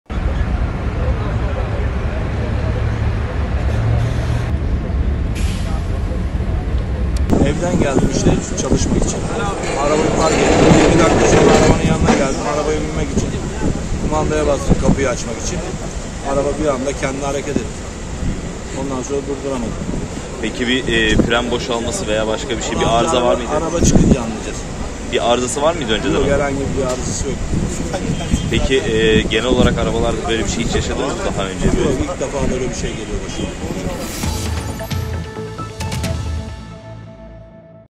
İstanbul Eminönü'nde İETT otobüsü denize düştü | Yaşanan olayı şoförü anlattı
İstanbul Eminönü'nde park halindeki boş İETT otobüsü kayarak denize düştü.

Ragıp Gümüşpala Caddesi'nde otobüs duraklarında park halinde bulunan bir İETT otobüsü, henüz belirlenemeyen nedenle kaymaya başladı. Denize düşen otobüs bir süre sonra suya gömüldü.

İhbar üzerine olay yerine itfaiye, sahil güvenlik ve polis ekipleri sevk edildi.

İstanbul Büyükşehir Belediyesine (İBB) ait vinçler de çalışmalara destek vermek amacıyla bölgeye gönderildi.

Ekipler, otobüsü denizden çıkarmak için çalışma başlattı.

Düşen otobüsün şoförü Ramazan Şahin, gazetecilere yaptığı açıklamada, otobüsü park ettikten 20 dakika sonra aracın yanına geldiğini söyledi.

Şahin, "Kumandaya bastım kapıyı açmak için bir anda araç kendi hareket etti, ondan sonra durduramadık. Herhangi bir arızası yoktu. İlk defa böyle bir şey geliyor başıma. Araç denize giderken arkadaşlarla gördük." dedi.